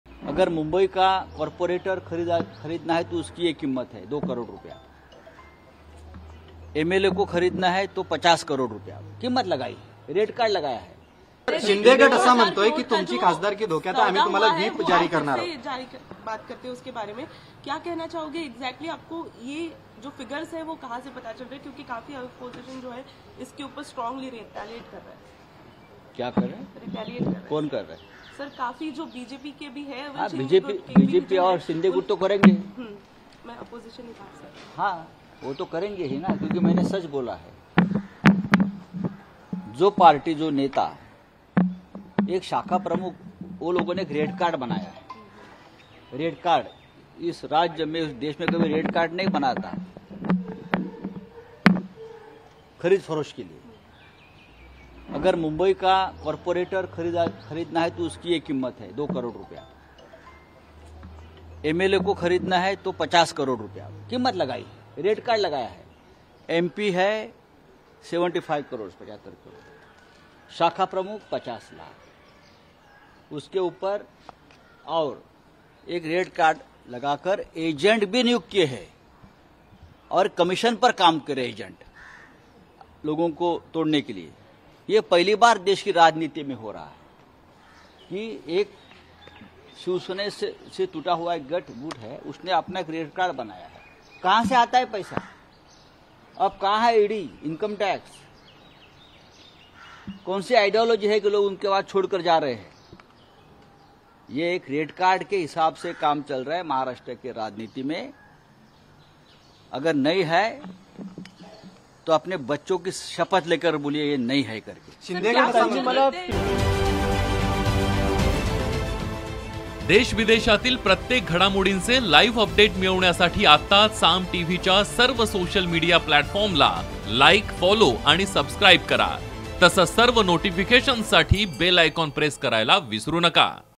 शिंदे गट विरुद्ध ठाकरे गट कलगीतुरा,Sanjay Raut यांचे शिंदे गटावर गंभीर आरोप | SAAM TV
राज्यात शिंदे गट विरुद्ध ठाकरे गट असा कलगीतुरा सुरू असताना दोन्ही बाजूंनी एकमेकांवर गंभीर आरोप केले जात आहेत. एकीकडे सर्वोच्च न्यायालयात राज्यातील सत्तासंघर्षावर सुनावणी होत असताना दुसरीकडे राज्यात मात्र राजकीय वातावरण चांगलंच तापलं आहे. ठाकरे गटाचे खासदार संजय राऊत यांनी गेल्या दोन दिवसांपासून दोन हजार कोटींच्या ‘पॅकेज’बाबत दावा करून चर्चेची राळ उडवून दिली आहे. त्यापाठोपाठ आज संजय राऊतांनी माध्यमांशी बोलताना थेट रेटकार्डच जाहीर केलं आहे. शिंदे गटाकडून ठाकरे गटातील आमदार-खासदार आणि इतर पदाधिकाऱ्यांना खरेदी करण्यासाठी किती रुपये दिले जात होते, याचा दावा संजय राऊतांनी केला आहे.